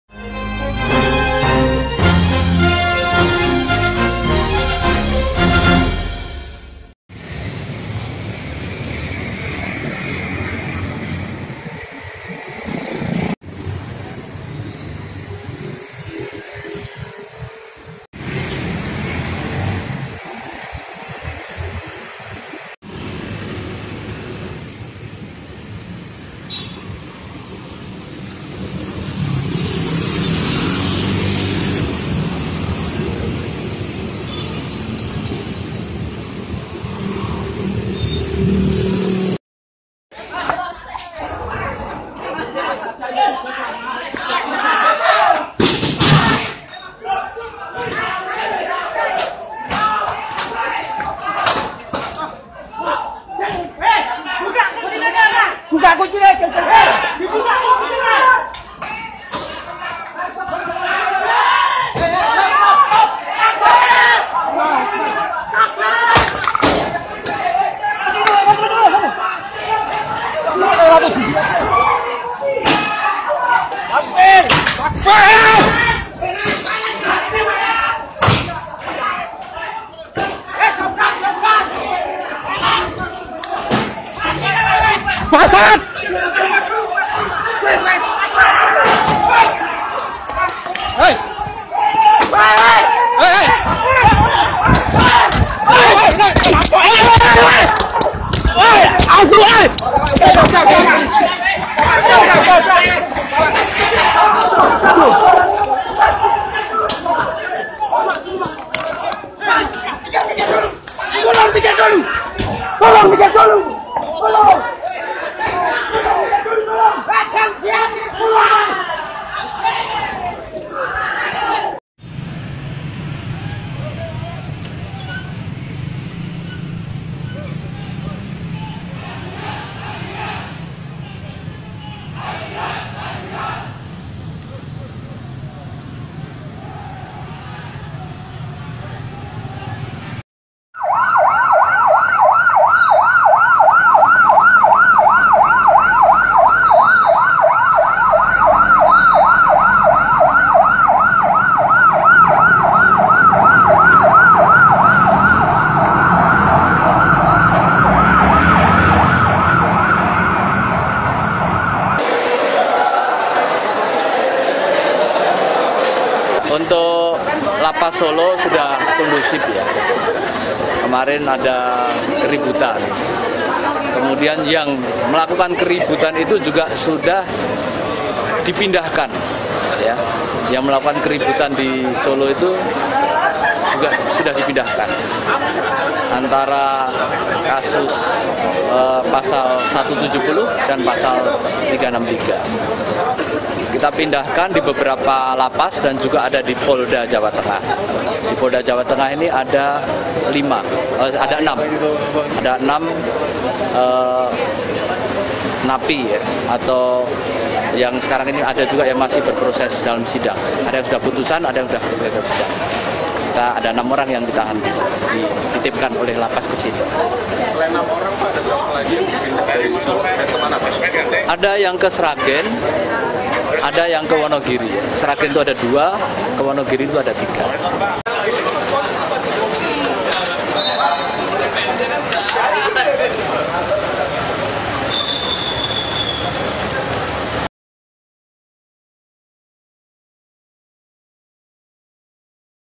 KONDISI RUTAN SOLO PASCA INSIDEN DUA KELOMPOK
Polda Jawa Tengah memastikan situasi Rumah Tahanan Negara (Rutan) Kelas I Solo dalam kondisi aman. "Situasi Rutan Kelas I Solo saya pastikan aman dan kondusif pascakeributan yang terjadi kemarin. Para napi yang terlibat keributan sudah dipindahkan ke beberapa lapas yang ada di Jateng, termasuk di Polda. Condro menerangkan dari total 11 napi yang dipindah, saat ini, ada enam narapidana yang dititipkan di ruang tahanan Mapolda Jateng. Sedangkan lima lainnya, masing-masing dua di Lembaga Pemasyarakatan (LP) Sragen dan tiga di LP Wonogiri. Menurutnya, hingga kini, tidak ada penetapan tersangka pascakeributan. Pasalnya, ricuh yang terjadi di Rutan Solo itu merupakan buntut dari terprovokasinya dua kelompok tahanan.

Dikatakannya, penyebab keributan yang terjadi di dalam Rutan Kelas I Solo saat ini masih dalam penyelidikan oleh kepolisian. Pihaknya juga melakukan antisipasi dengn mempertebal pengamanan. Selain itu, Condro mengaku telah bertemu dengan Kakanwil Kemenkumham Jateng untuk berkoordinasi terkait pengamanan di masing-masing LP. “Keamanan lapas diperketat, satu regu dari masing-masing polres, termasuk juga pengunjung juga lebih diperketat,” terangnya. Terkait pemindahan belasan napi, dalam proses menunggu persidangan. “Begitu sidang dan putusannya nanti menunggu pengadilan, jadi ini kan masih dalam proses penanganan ini di kejaksaan,” katanya.